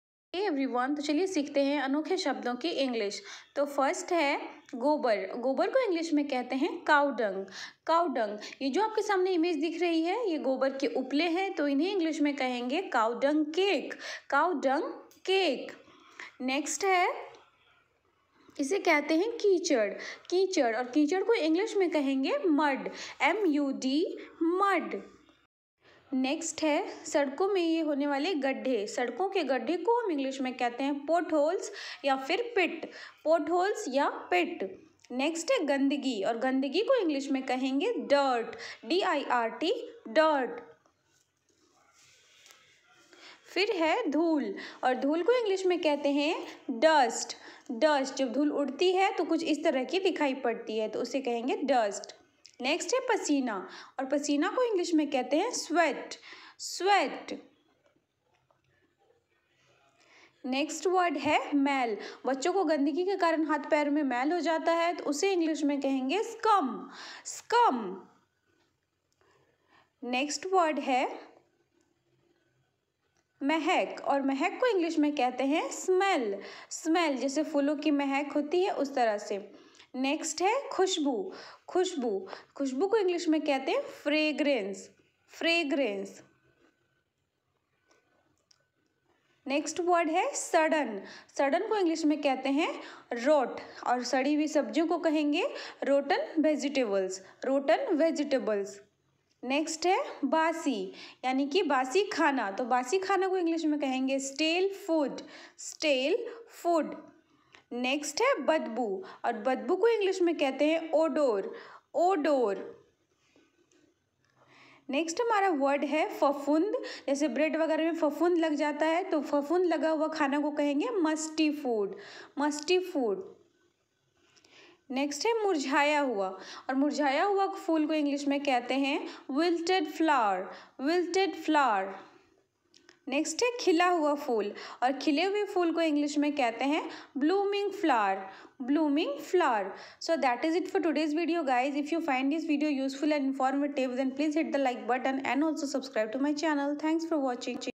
अनोखे शब्दों की इंग्लिश।Improve your vocabulary//Common hindi to English word meaning
घर पर रोज बोले जाने वाले इंग्लिश

गोबर को इंग्लिश में क्या कहते हैं
बदबू को इंग्लिश में क्या कहते है
सड़न को इंग्लिश में क्या कहते है।